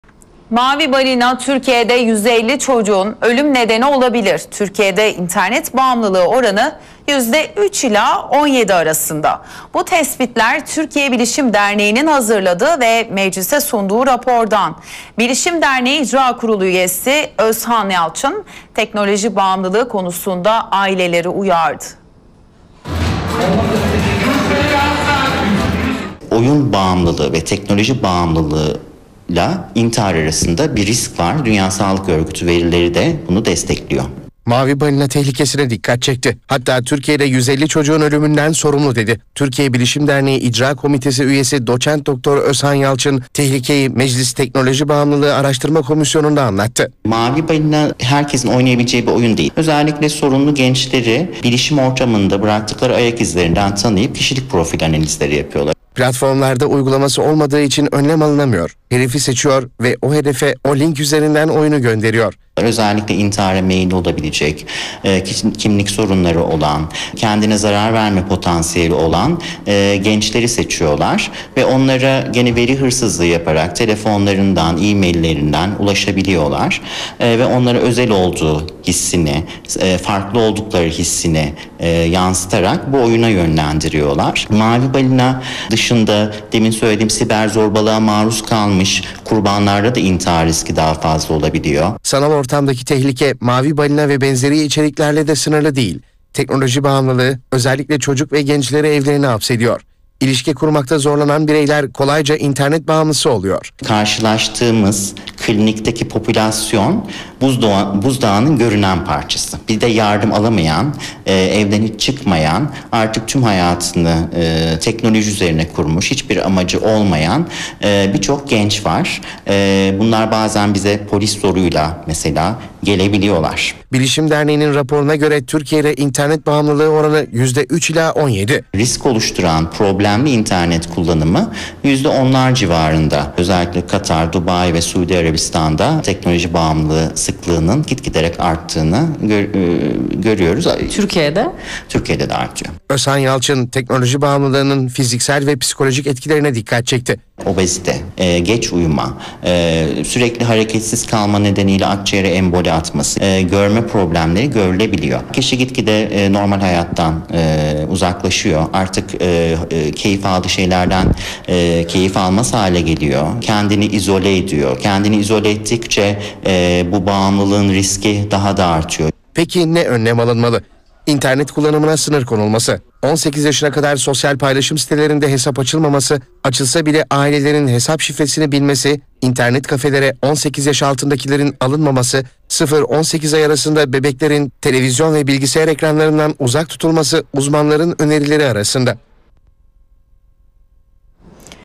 MAVİ BALİNA MECLİS GÜNDEMİNDE - HABERTÜRK
Türkiye'de internet bağımlılığı oranı 3% ile 17% arasında. Bu tespitler Türkiye Bilişim Derneği'nin hazırladığı ve meclise sunduğu rapordan. Bilişim Derneği İcra Kurulu üyesi Özhan YALÇIN teknoloji bağımlılığı konusunda aileleri uyardı.